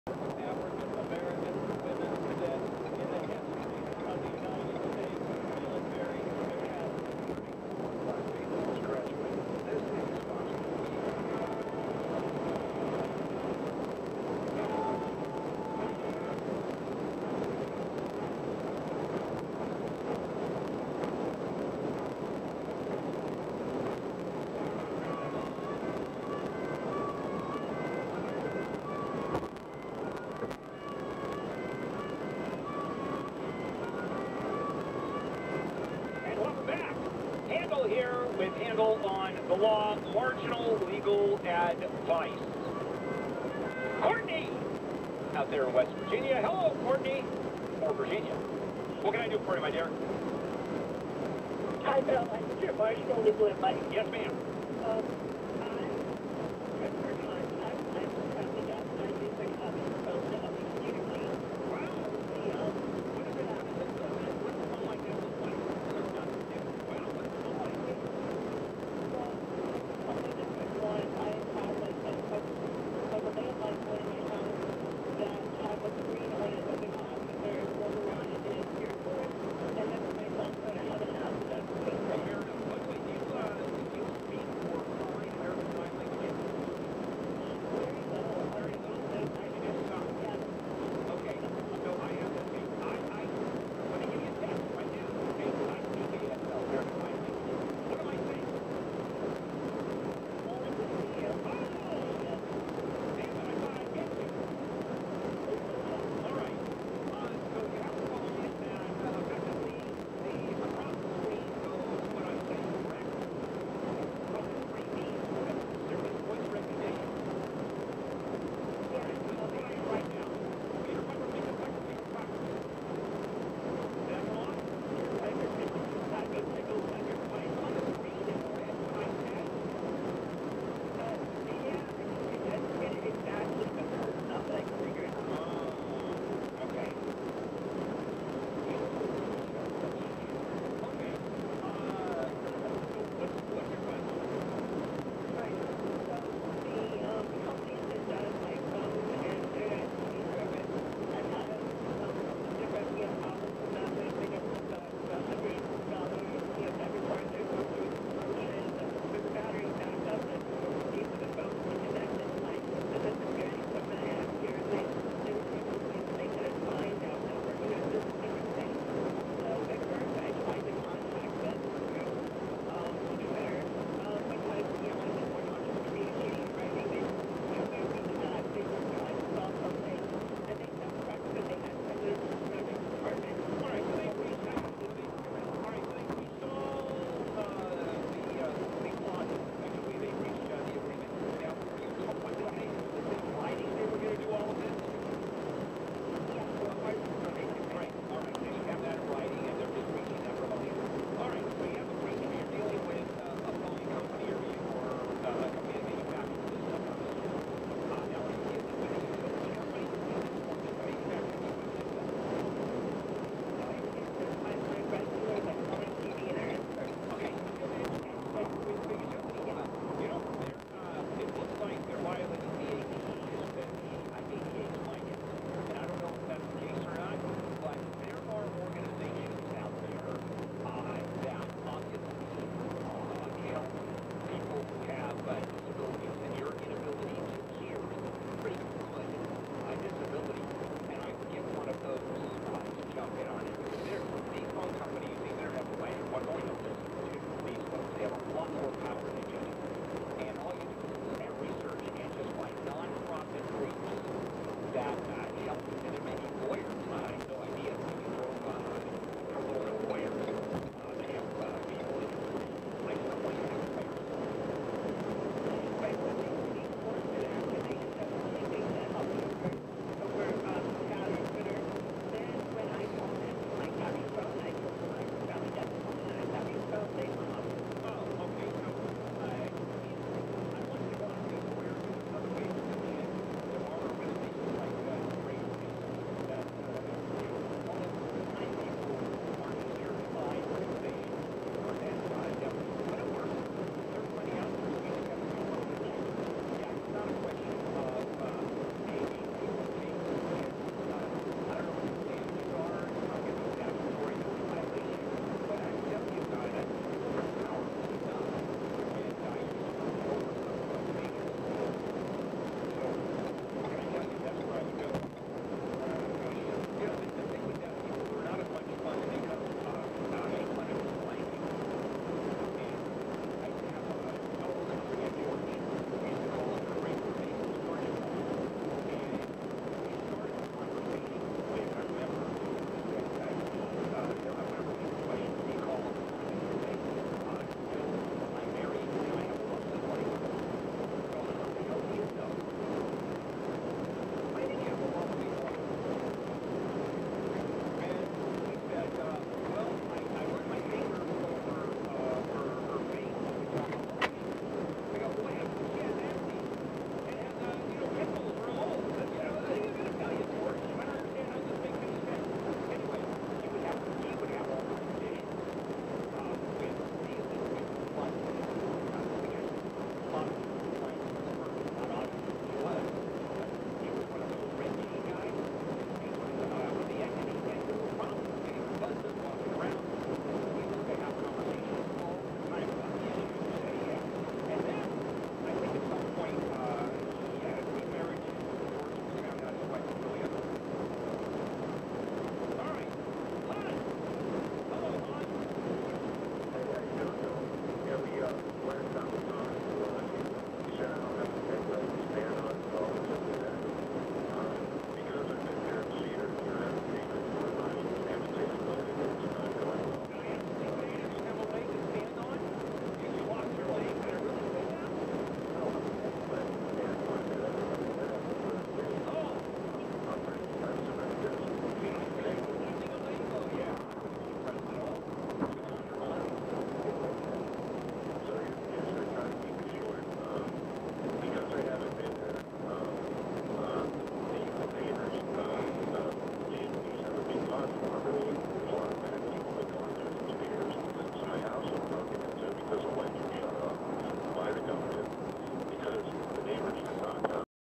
Interstate 555 (Exits 18 to 29) northbound
Interstate 555 is a freeway spur connecting I-55 near West Memphis with Jonesboro and points north along the US Route 63 corridor.